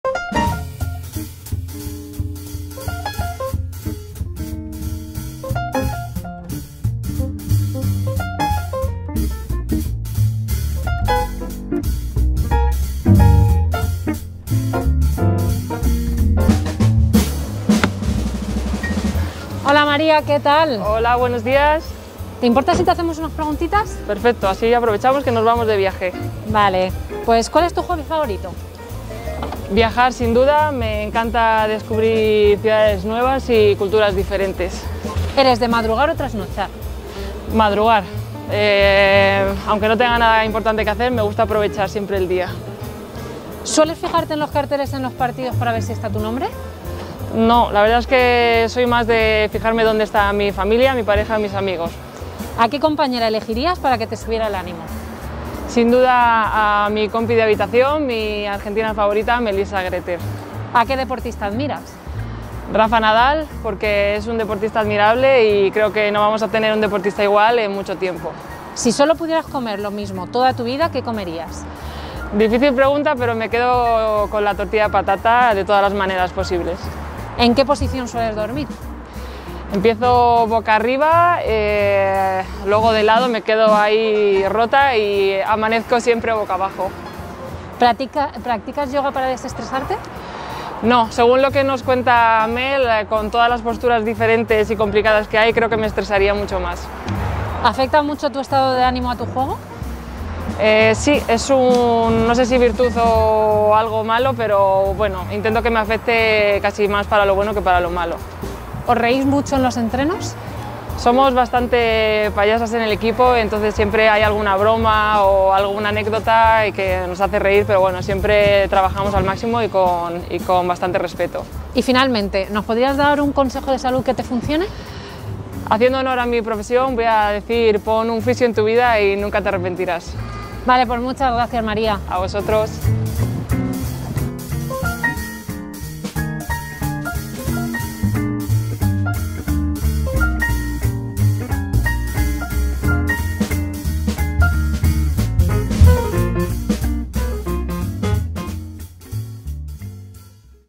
Con ASISA, 10 preguntas y un consejo de salud de María Espín
La capitana de las WomenInBlack de Movistar Estudiantes, María Espín, responde a 10 preguntas personales y nos da un tip de salud de la mano de @GrupoAsisa

Viajar, Rafa Nadal, comida favorita, costumbres al dormir, el buen rollo en los entrenamientos...
💪 Como consejo, PON UN FISIO EN TU VIDA